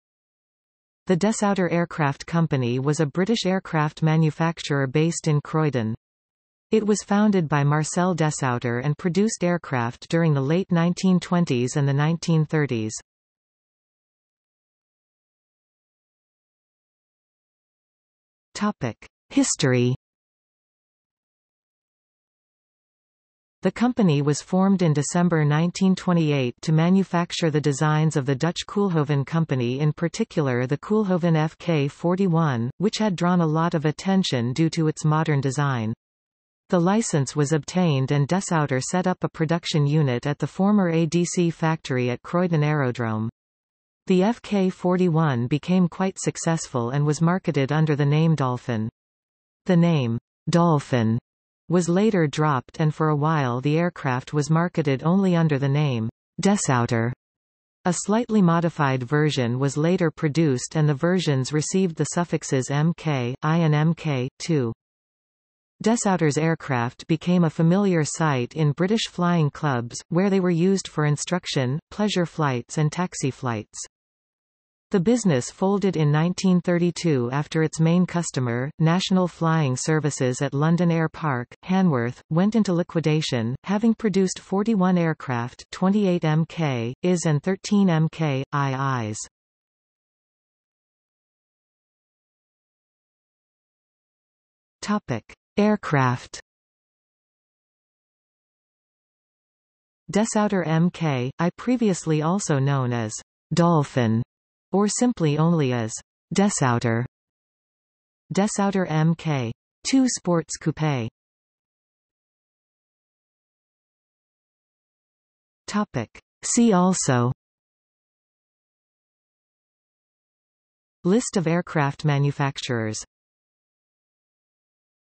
Desoutter Aircraft Company | Wikipedia audio article
00:00:14 1 History
00:01:31 2 Aircraft
00:01:52 3 See also

- Socrates

SUMMARY
The Desoutter Aircraft Company was a British aircraft manufacturer based in Croydon. It was founded by Marcel Desoutter and produced aircraft during the late 1920s and the 1930s.